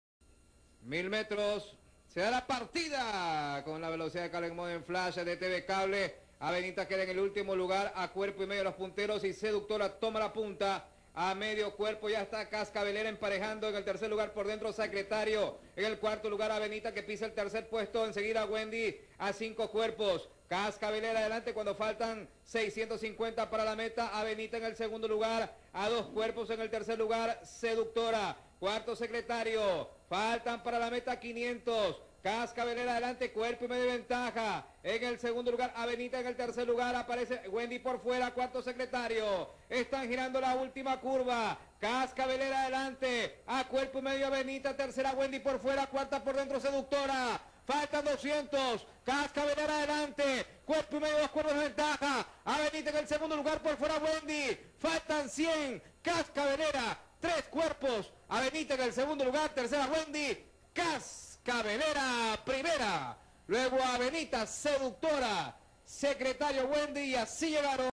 2010-181 Cascabelera (Ecu) - ago.22.2010 - Revista La Fija
VIDEOS DE REVISTA LA FIJA

Hipódromo Miguel Salem Dibo (Buijo); Guayaquil - Ecuador;
CASCABELERA (Ecu-2005), hembra alazana por Devil's Moon y Hermana Luna (Niobrara); Décima victoria en 45 salidas. Distancia: 1.000 metros; Tiempo: 1'00"4; Jinete: Manuel González Jr. (Ecu), 56 kg.; Preparador: Hernando Díaz (Col); Stud: Topito.

Posiciones finales:
1º Cascabelera (Ecu);
2º Avenita (Ecu) 3-1/2 cpos;
3º Seductora (Ecu) 5 cpos;
4º Secretario (Ecu) 5 cpos + cab.;
5º Wendy (Ecu) 6 cpos.